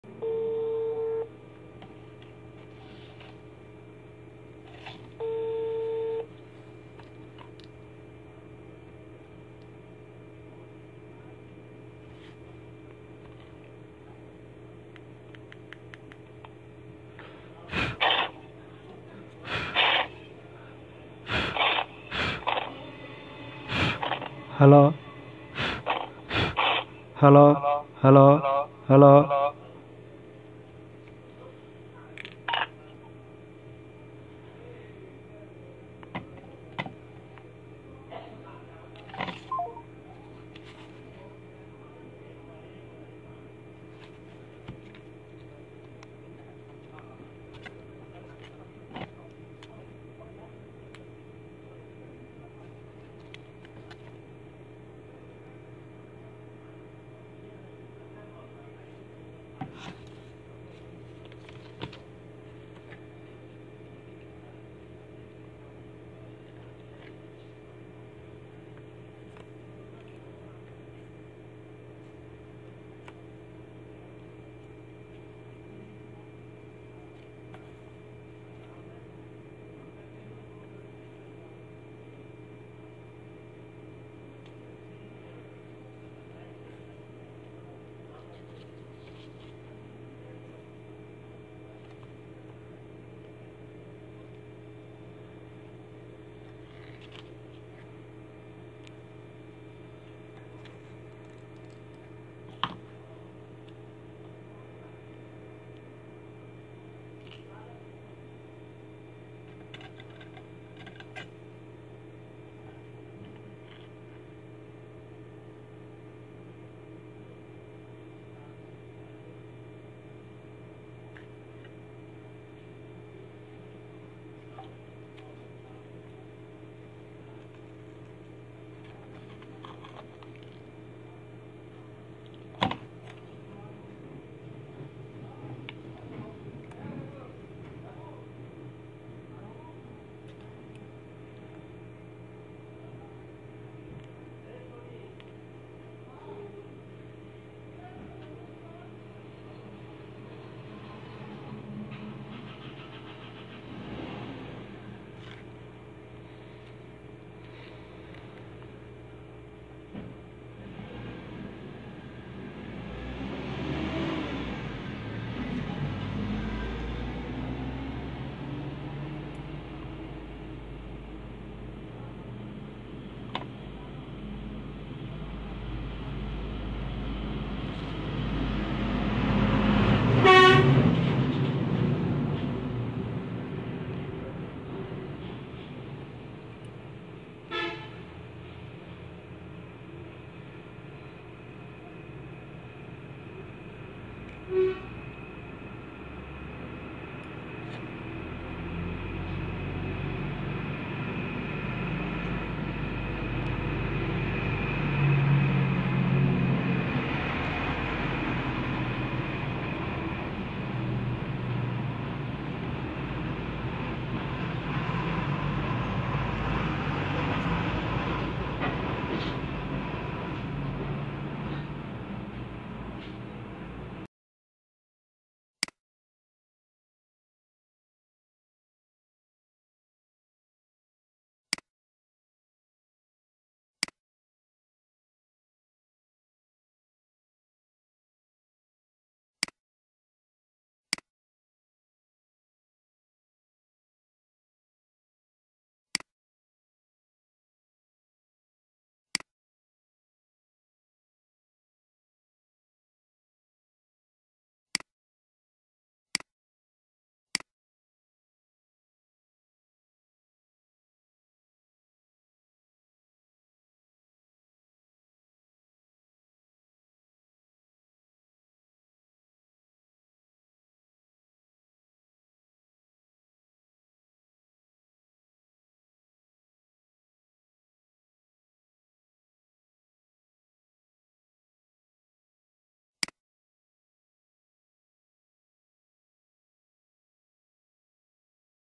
Samsung j210f broken board repair. mic charging and network jumper solution
(((Gsmbd24)) চ্যানেলে আপনাকে স্বাগতম ।